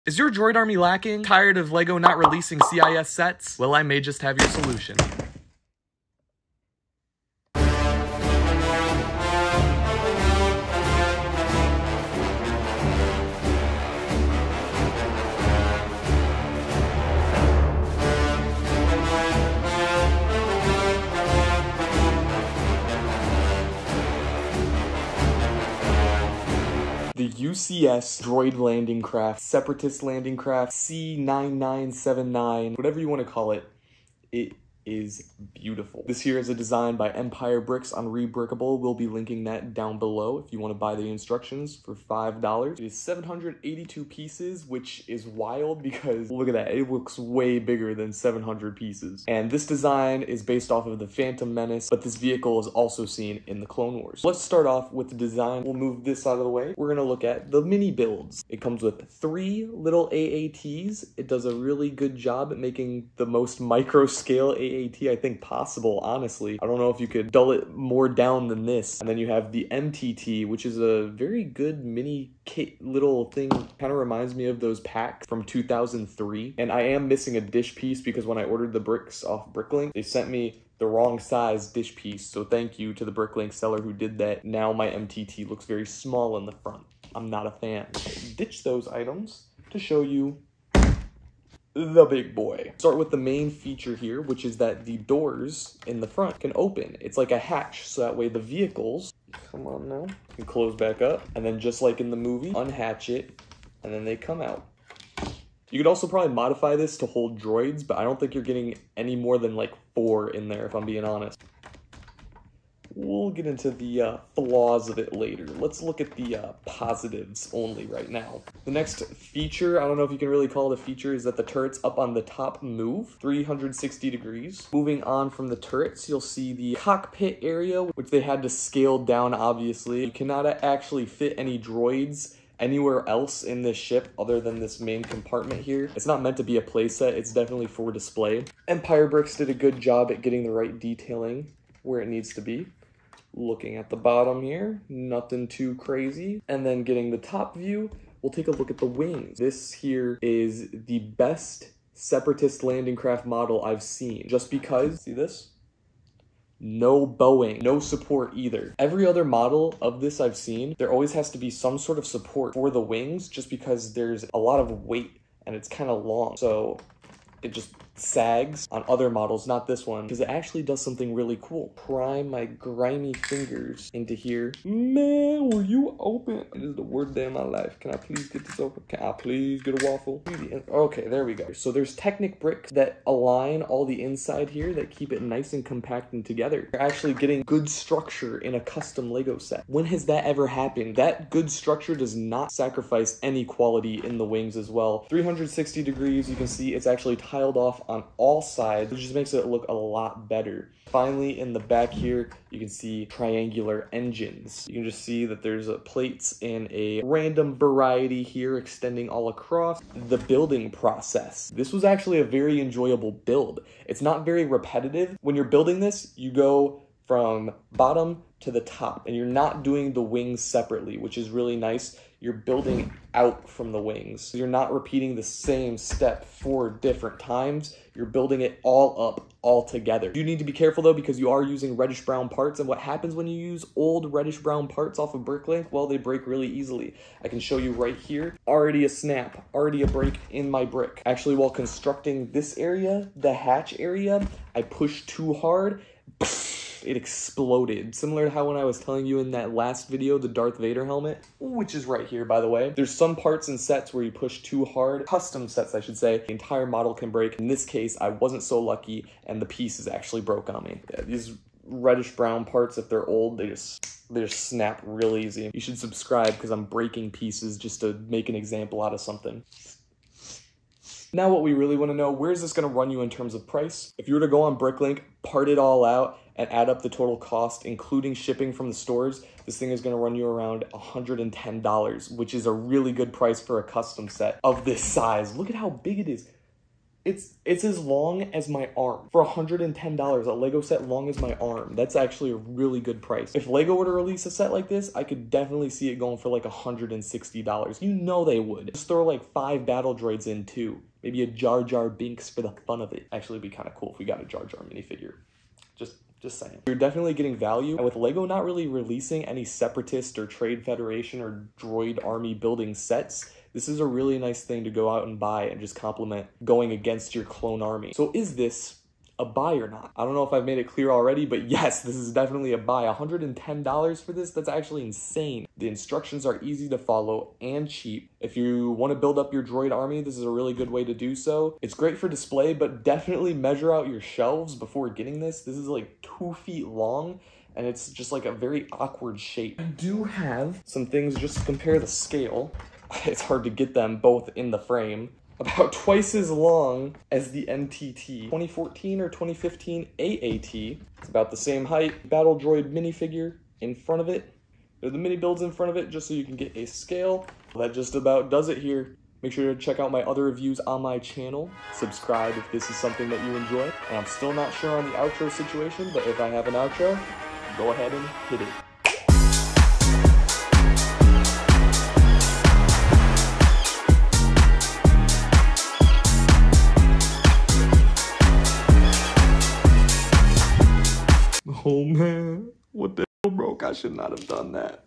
YOU CAN AFFORD THIS - Custom Droid Landing Craft Review
Here's my review of a custom Lego Star Wars Separatist Landing Craft. This set is huge and will help build up your droid army. Come see if it's worth the purchase.

Model and design by EmpireBricks on Rebrickable
Buy the instructions ! - UCS C-9979 Droid Landing Craft (Trade Federation) by EmpireBricks on Rebrickable

Add me on Twitter - @StarFishNext